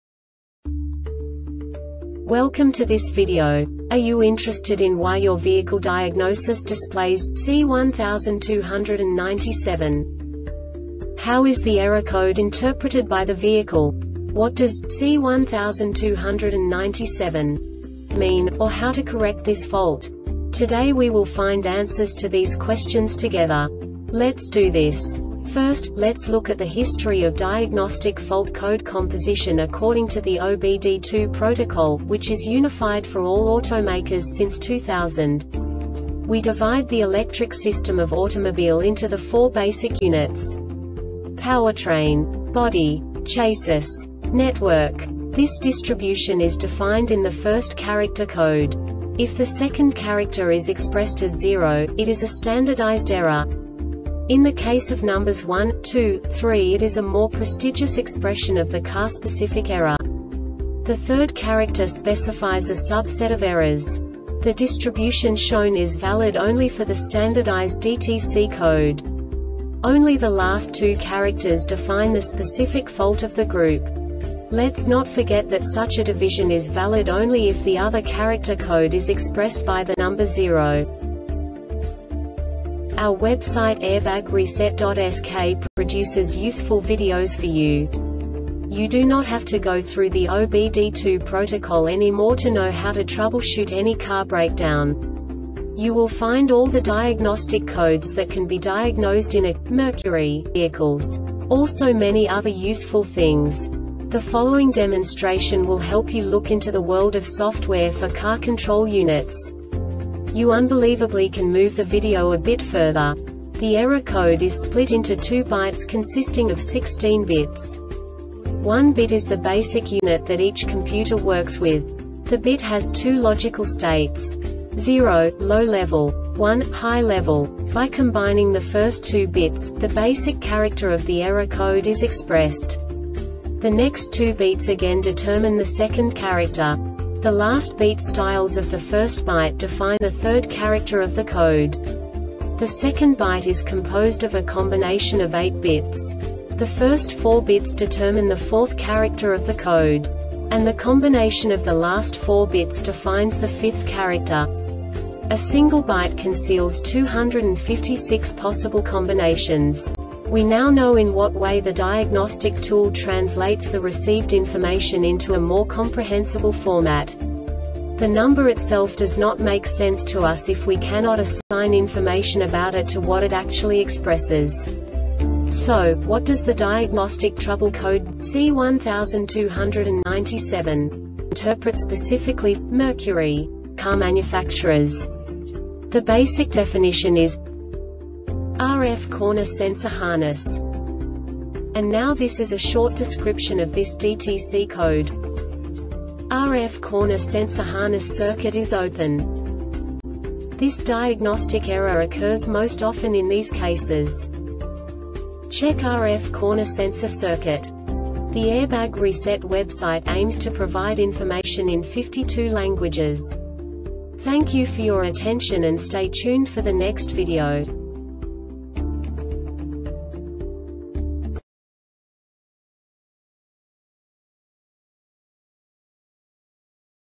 DTC Mercury C1297 Short Explanation
Contents:
0:21 Basic DTC analysis according to OBD2 protocol standard.
1:48 Insight into programming
2:58 A diagnostically understandable description of the Diagnostic Trouble Code
3:05 Basic error definition

Make
Mercury
Code
C1297
Definition
RF Corner Sensor Harness
Description
RF corner sensor harness circuit is open
Cause
Check RF corner sensor circuit